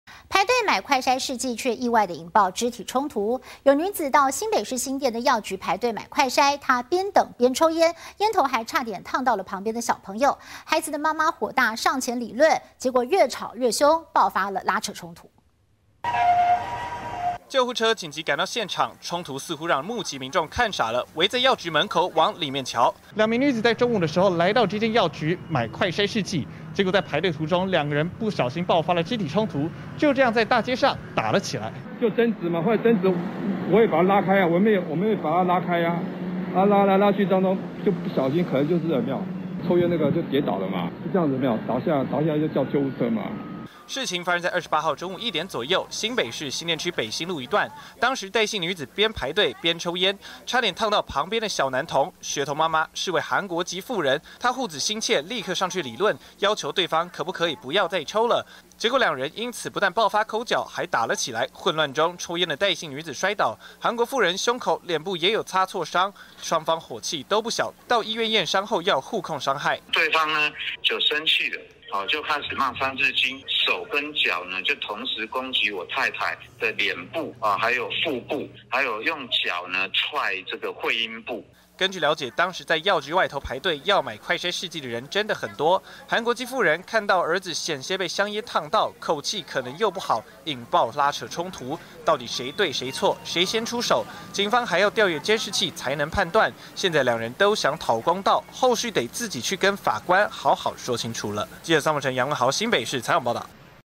韓籍婦攜子排隊買快篩 女一旁抽菸引爆衝突
排隊買快篩試劑，卻意外爆發肢體衝突! 有女子到新北市新店的藥局排隊買快篩，她邊等邊抽菸，菸頭還差點燙到旁邊的學童，小孩媽媽護子心切，火大上前理論，結果越吵越兇，爆發拉扯衝突，兩人雙雙掛彩，互控對方傷害，要上法院來解決。